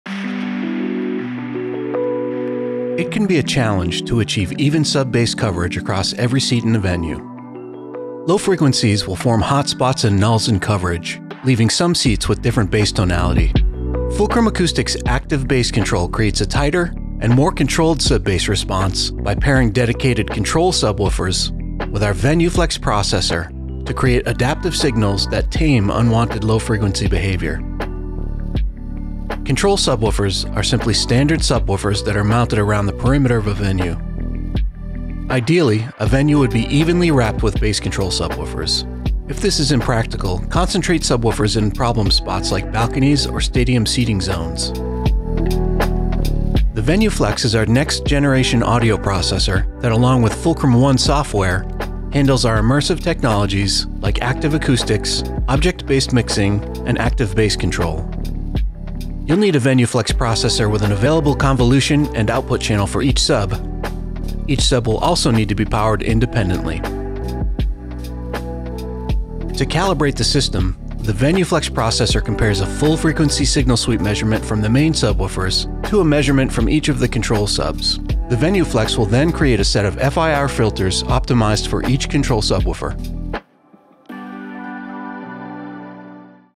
Active Bass Control Explained - Venueflex Processor Technology
Fulcrum Acoustic's Active Bass Control corrects for common low-frequency challenges in rooms like the appearance of power alleys and coverage nulls.

To achieve this, our Venueflex processors continually sample the subwoofer drive signals from an installed audio system, and produces complementary and adaptive signals to drive low-frequency control loudspeakers distributed around the room. This creates tighter, more controlled sub-bass response for an improved audio experience.

TRANSCRIPT
Achieving even sub-bass coverage across every seat in a venue can be challenging. Low frequencies often form hotspots and nulls in coverage, leaving some seats with inconsistent bass tonality. Fulcrum Acoustic’s Active Bass Control creates a tighter and more controlled sub-bass response by pairing dedicated “control” subwoofers with our Venueflex processor to generate adaptive signals that tame unwanted low-frequency behavior. Control subwoofers are standard subwoofers mounted around the venue’s perimeter. Ideally, the venue would be evenly wrapped with these subwoofers, but if that’s impractical, they can be concentrated in problem areas, such as balconies or specific stadium seating zones. The Venueflex is our next-generation audio processor that, along with Fulcrum One software, handles immersive technologies like Active Acoustics, Object-Based Mixing, and Active Bass Control. For each subwoofer, you’ll need a Venueflex processor with an available convolution and output channel, and each subwoofer will require independent power. To calibrate the system, the Venueflex processor compares a full-frequency signal sweep measurement from the main subwoofers to measurements from the control subwoofers. It then creates a set of FIR filters optimized for each control subwoofer.